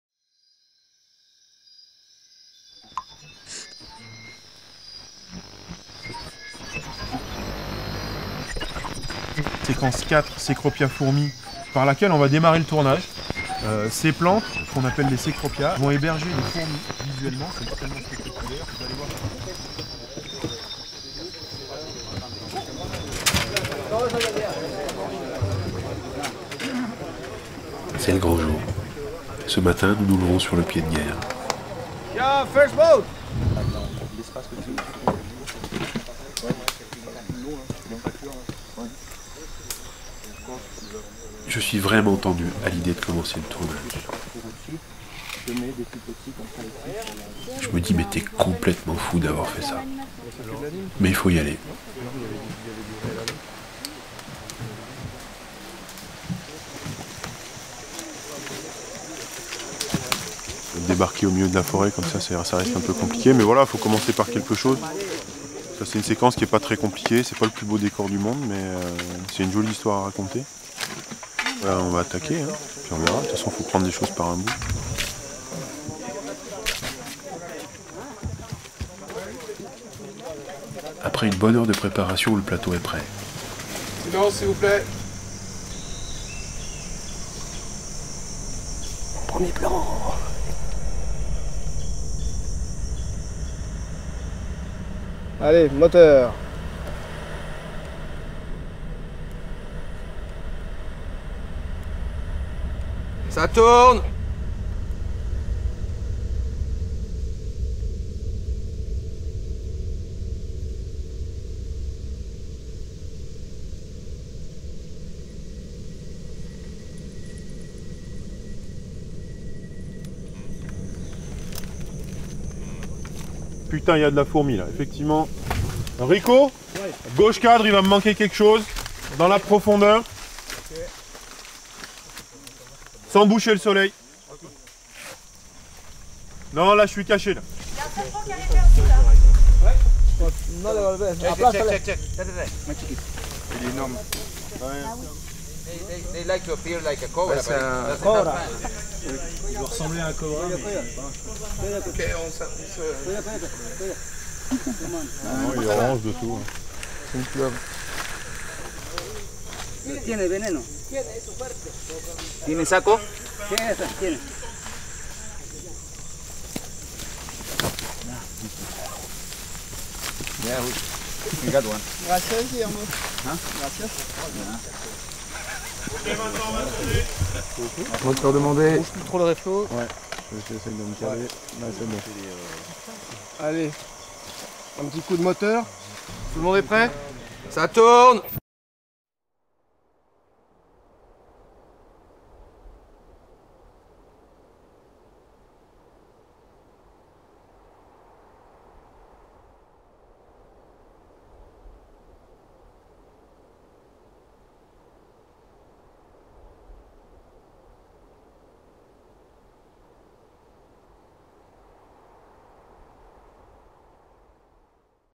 Premier jour de tournage - Voyages en forêt des pluies - Tournage #09 - Pérou
Après des années de préparation et de prospections forestières, le Grand Jour est arrivé.

Luc et son équipe tournent les tous premiers plans du film. Un grand moment d'émotion quelque peu perturbé par la visite surprise d'un habitant de la forêt... ACTION.